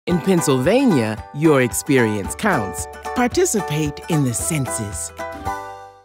2020 Census | Shape Your Future In PA
In Pennsylvania, your experience counts. We’re committed to working together to make sure all our neighbors are included in the census.